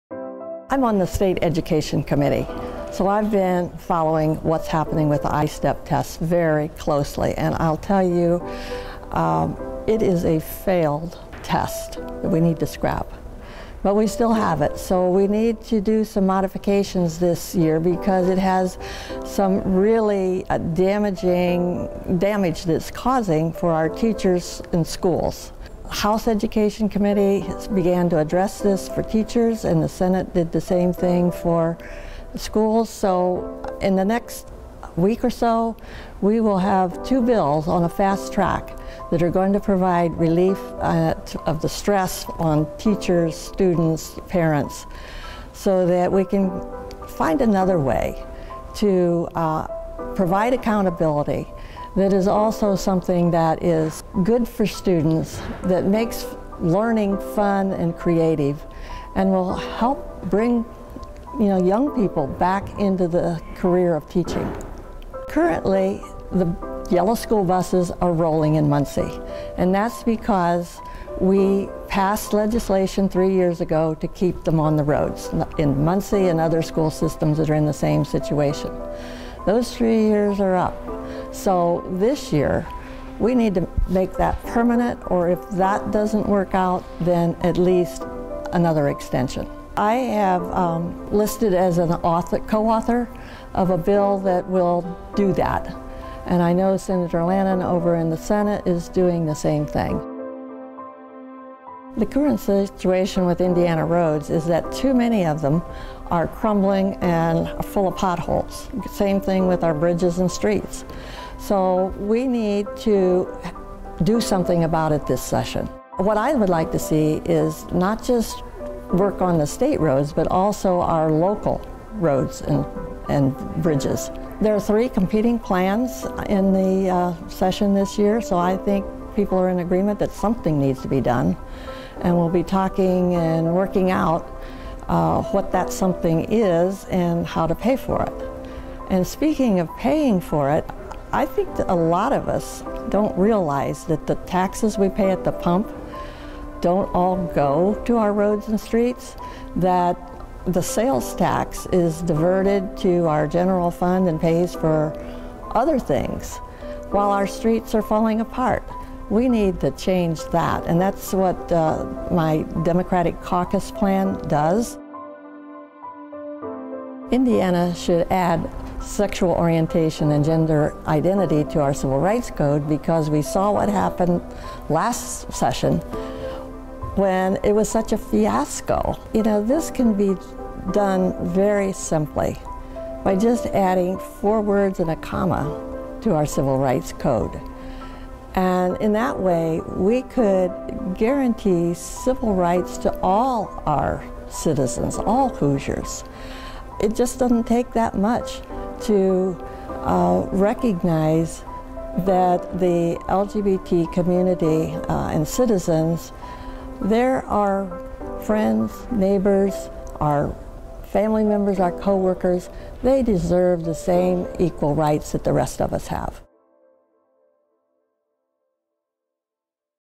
Rep. Sue Errington Discusses ISTEP, Infrastructure, School Transporation, and Civil Rights
Rep. Errington shares her thoughts on ISTEP, infrastructure, school transportation, and civil rights issues facing the General Assembly.